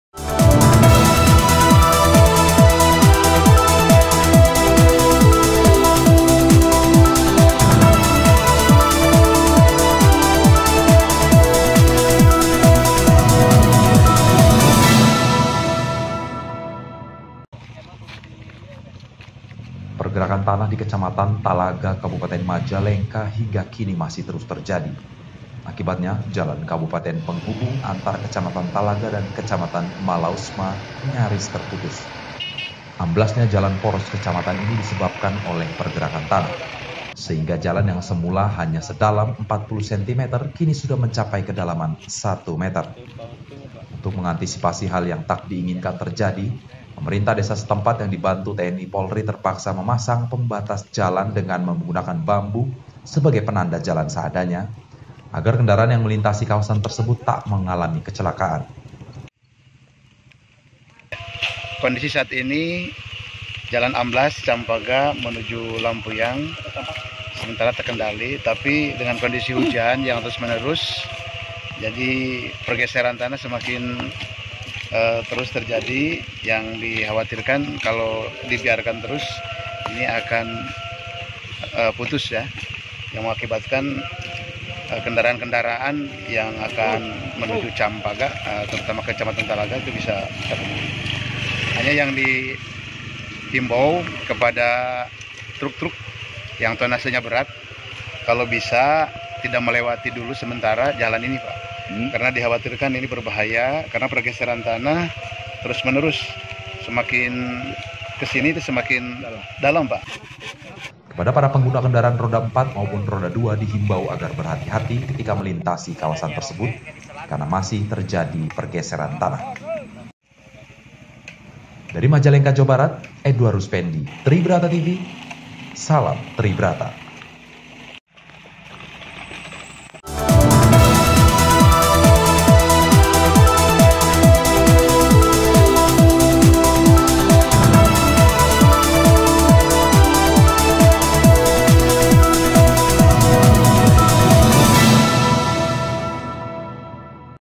JALAN KABUPATEN NYARIS TERPUTUS AKIBAT PERGERAKAN TANAH
PERGERAKAN TANAH DI KECAMATAN TALAGA KABUPATEN MAJALENGKA TERUS TERJADI HINGGA SENIN MALAM. AKIBATNYA JALAN KABUPATEN PENGHUBUNG ANTAR KECAMATAN TALAGA DAN KECAMATAN MALAUSMA NYARIS TERPUTUS.